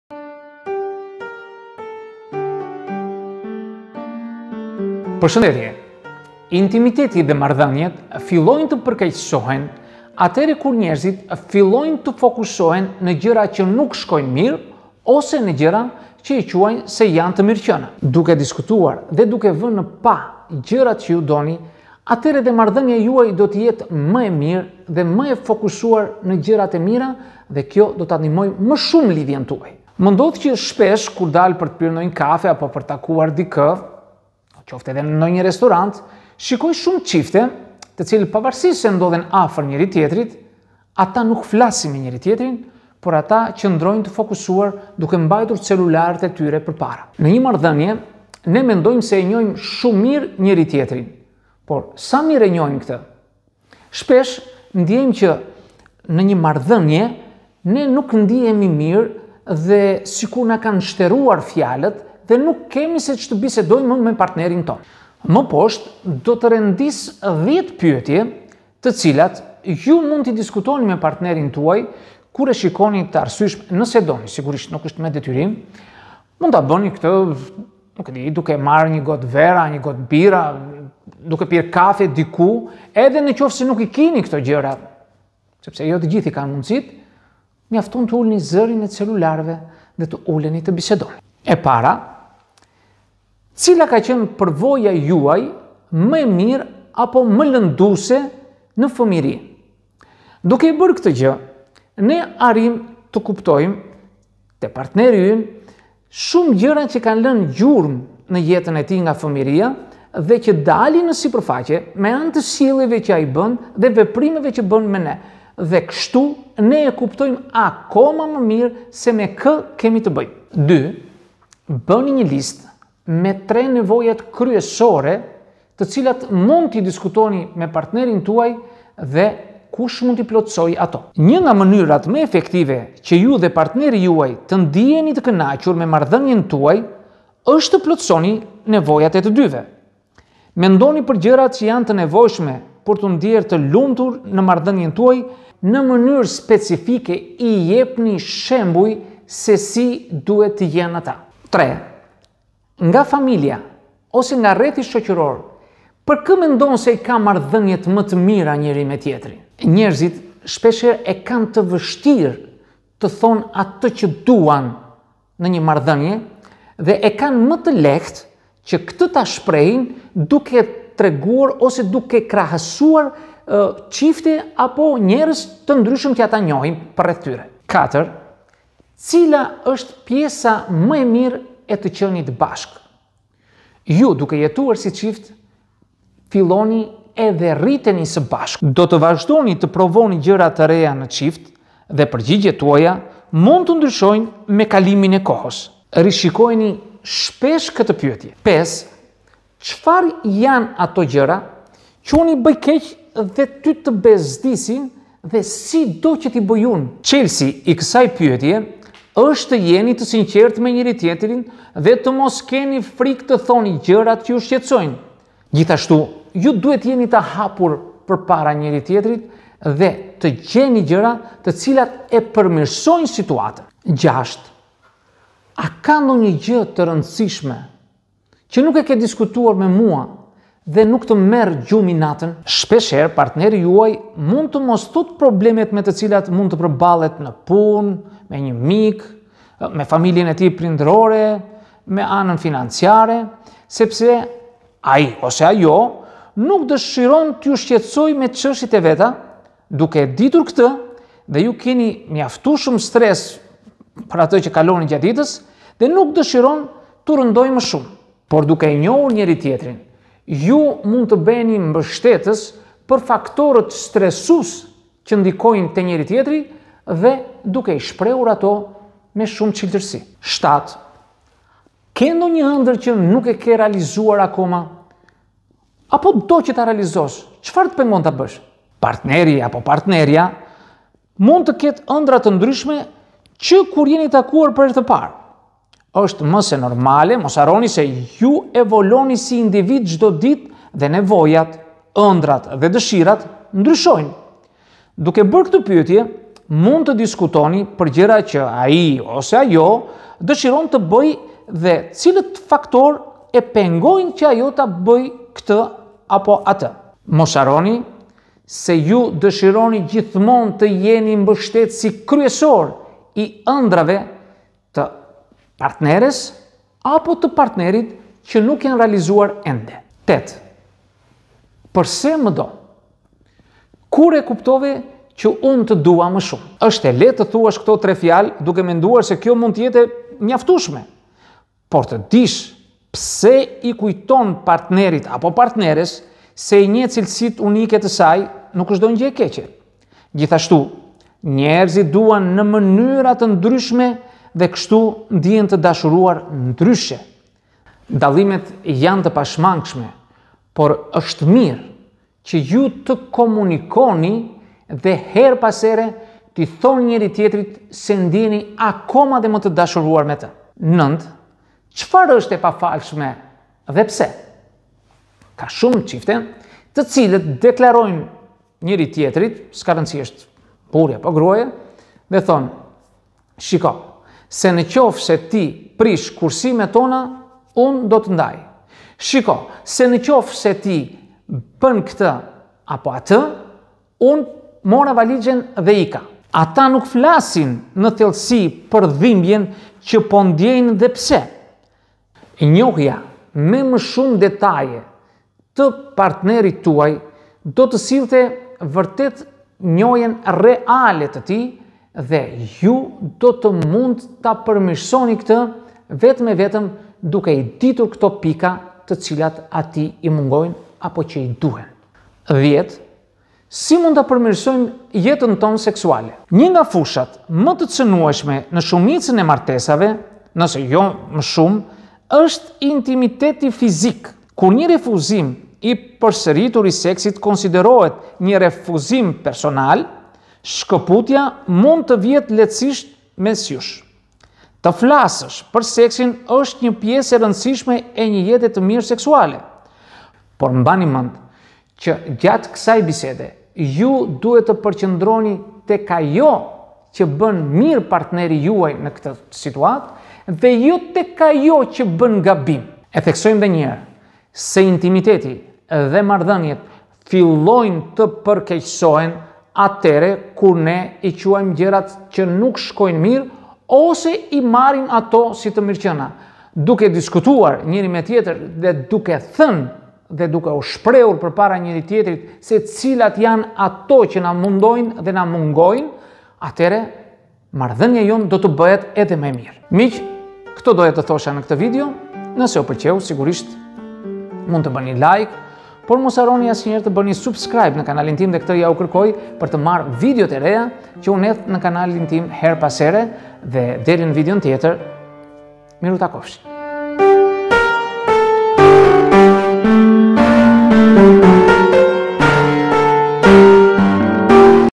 Si të afroheni me shumë me partnerin.
Intimiteti dhe marrëdhëniet fillojnë të përkeqësohen kur njerëzit qëndrojnë të fokusuar në gjëra që nuk i shikojnë mirë ose i marrin gjerat si të mirëqena. Në këtë video jo do të mësoni 10 pyetje të cilat mund t'i bëni partnerit tuaj dhe të përmirësoni jetën tuaj bashkëshortore.

Për me shumë dije shikoni videon

 Ju falënderoj!